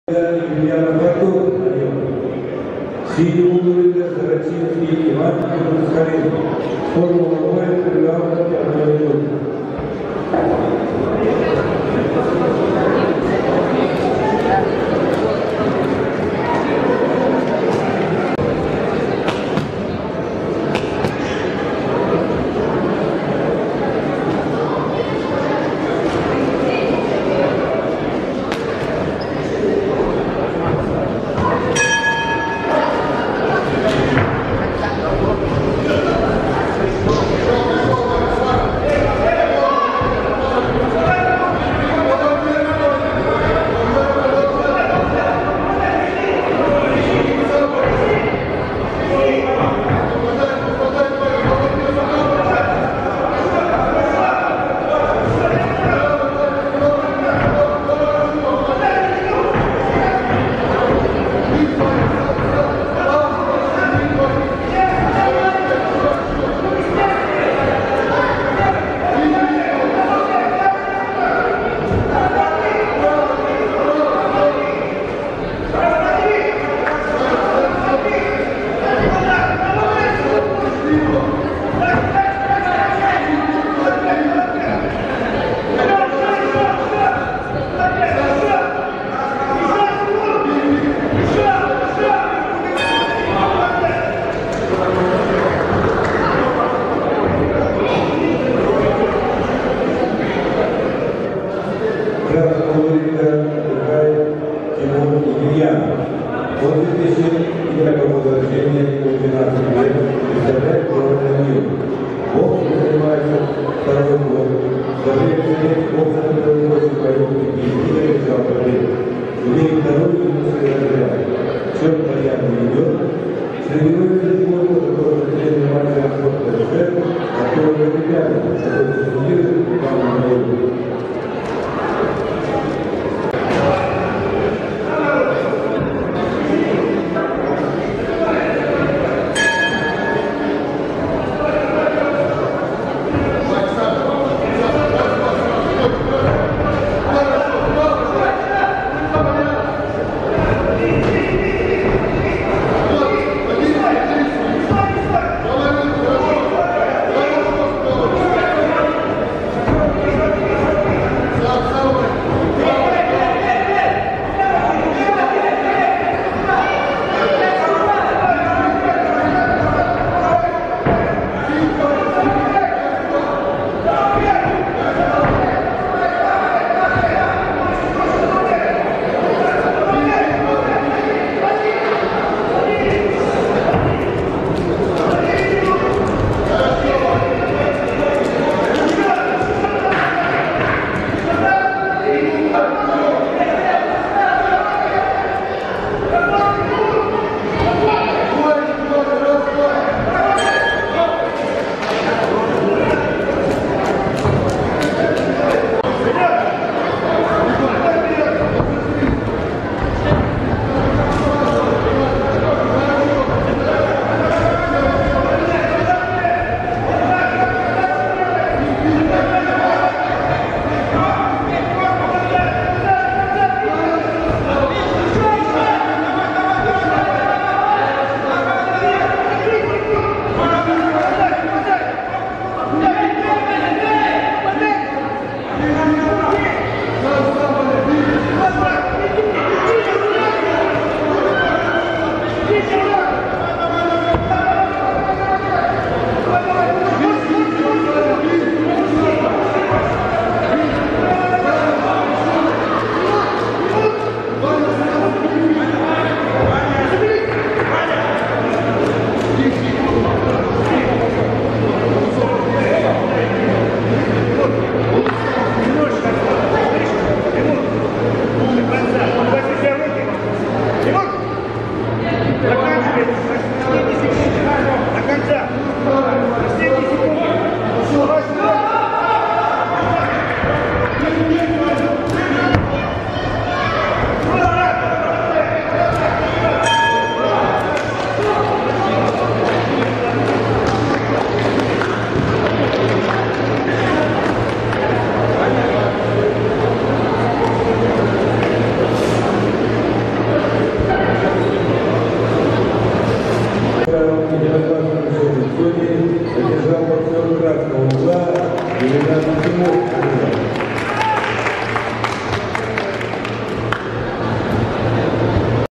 Емельянов Тимур(Анива) - Сорочинский Иван(Ю-Сах) (40кг).
Долинск-Бокс-(Четвертьфинал)-Памяти Е.Севастьянова и Д.Молостова(21 января 2021)